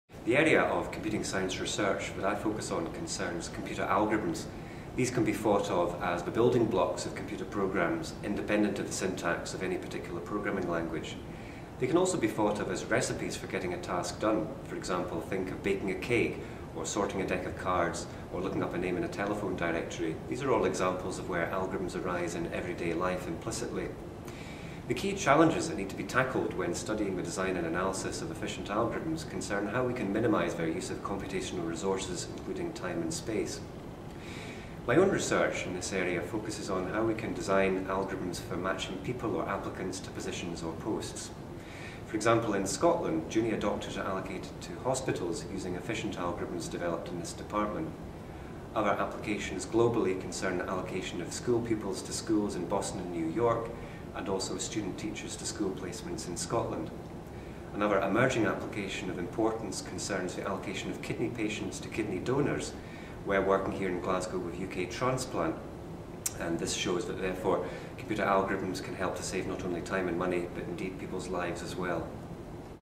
David Manlove
Lecturer at the Computing Science Department of Glasgow University.

Research interests lie mainly in the field of Algorithms and Complexity, and include:

    * Matching problems, including stable matching
    * Algorithmic graph theory, including colouring, independence and domination in graphs
    * Complexity and approximability of optimisation problems
    * Stringology